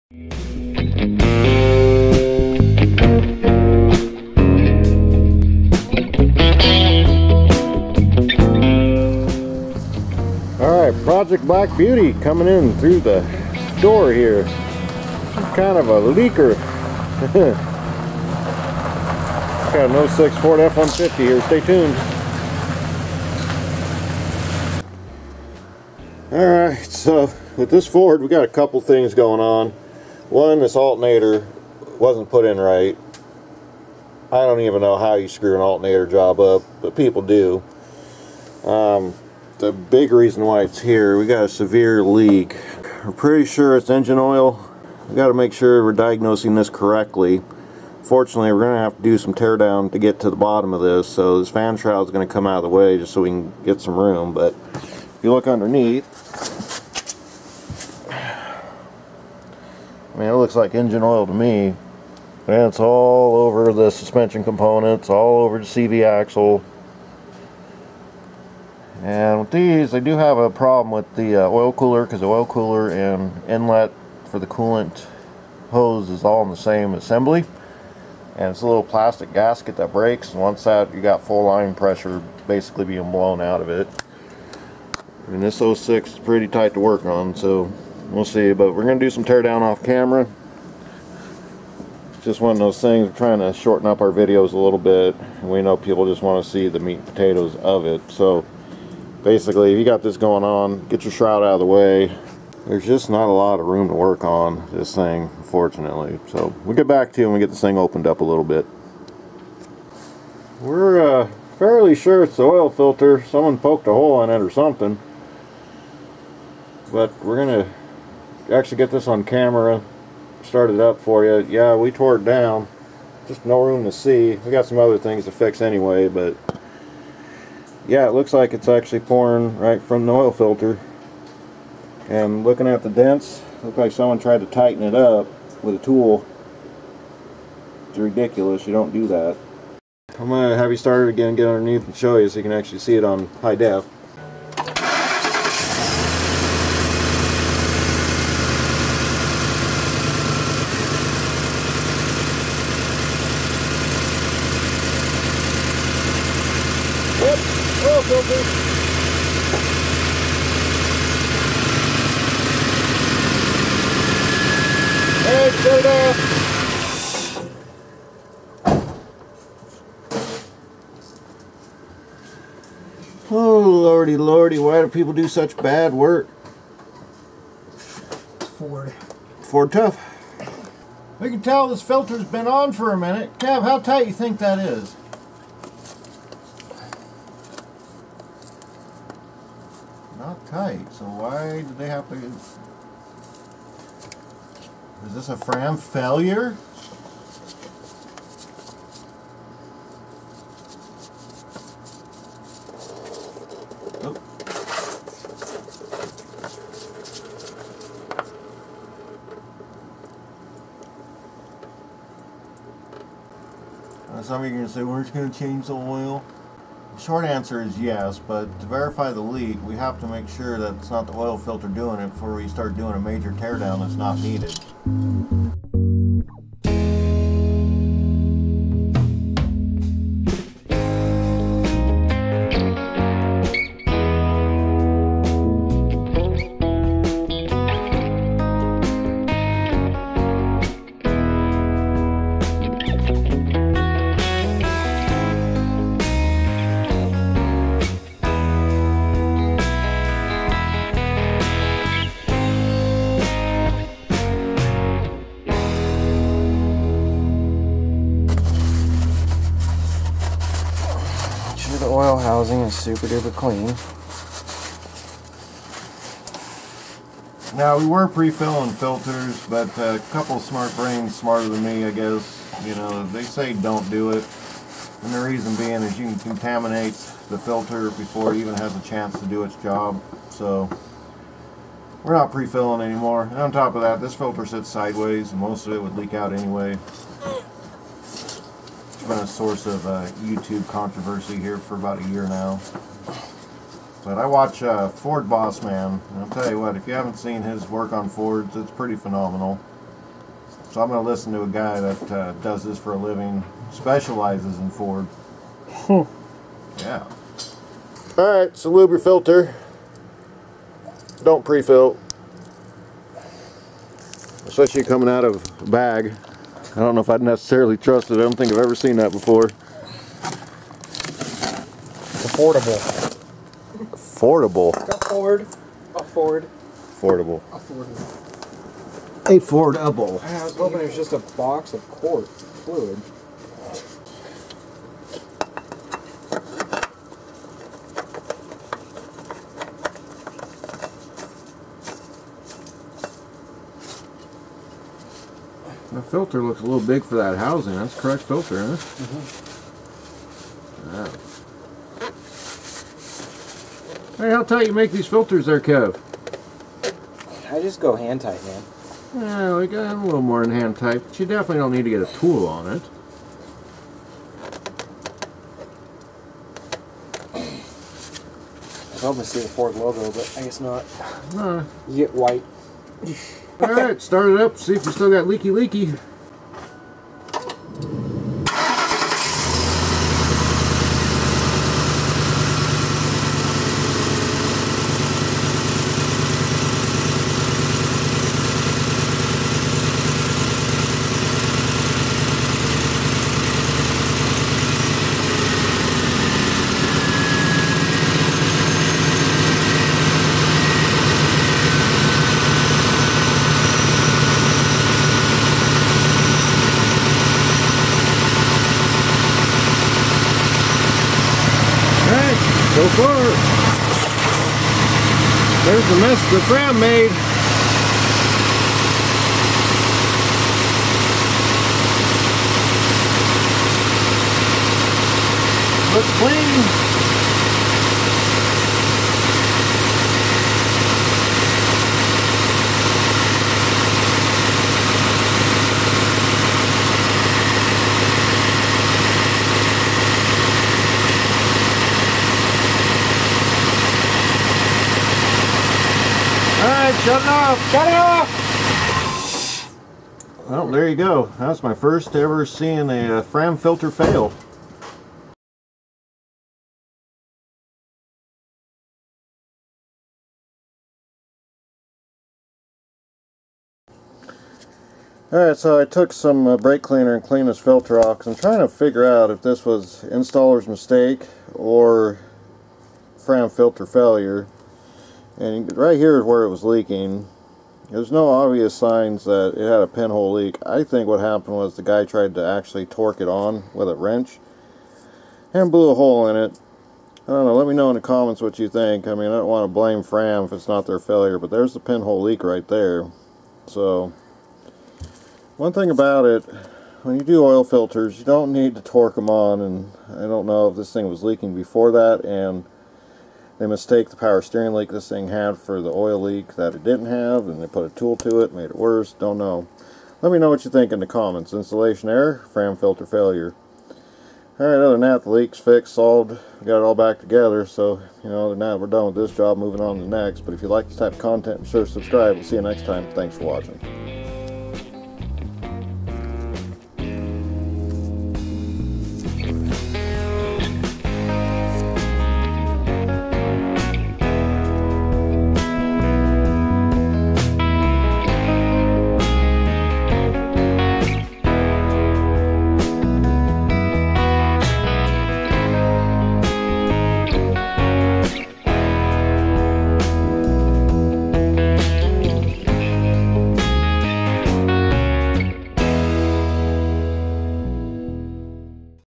Fram Oil Filter Failure???
This 2006 Ford F-150 came in the shop for a major oil leak.  After doing some diagnosis we found the Fram Tough Guard Oil Filter was leaking.  Was it a Fram oil filter failure or installers mistake? Watch the video to find out. Let us know what you think was the cause of this filter failure.